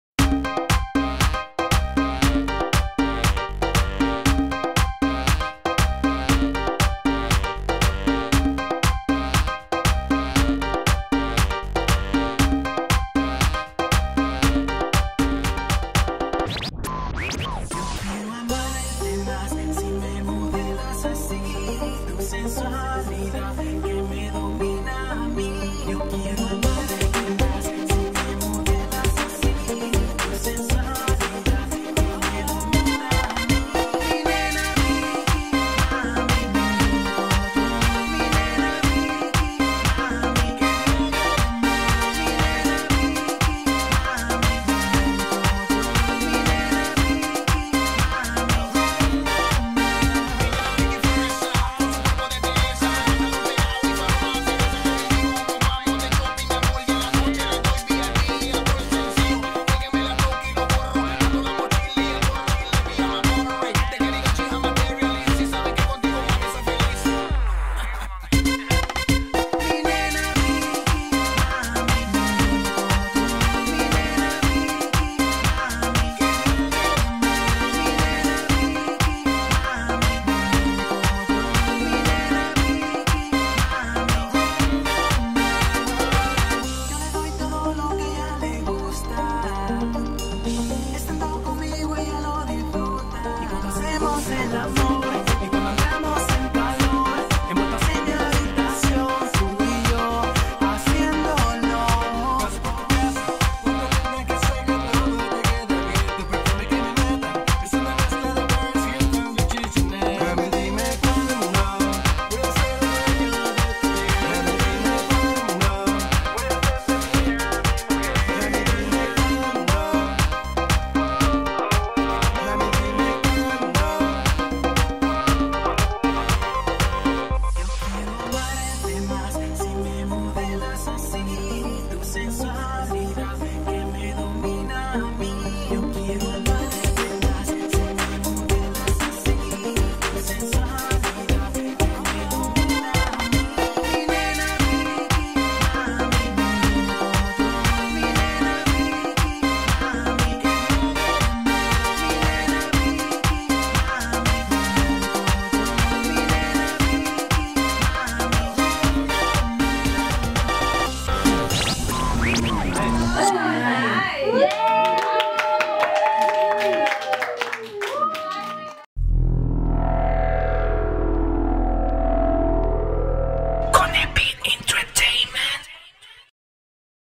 Mayerly Rodriguez / MGR BEHIND THE SCENES
Colombian Mayerly Rodriguez The Celebrities' Fashion Designer photo-shoot for the web page, in collaboration of the Talented professionals: From Indonesia Leo Gunawan, From El Salvador Conebit ENT, From Bolivia Jossett Valdez, From Mexico Palmira Fernandez, From Bolivia-Paraguay Mauricio Cuevas, From USA Steve Cesar,  From Colombia Gissell Botero, Models: Mexico, Nigeria, USA, Russia, Colombia.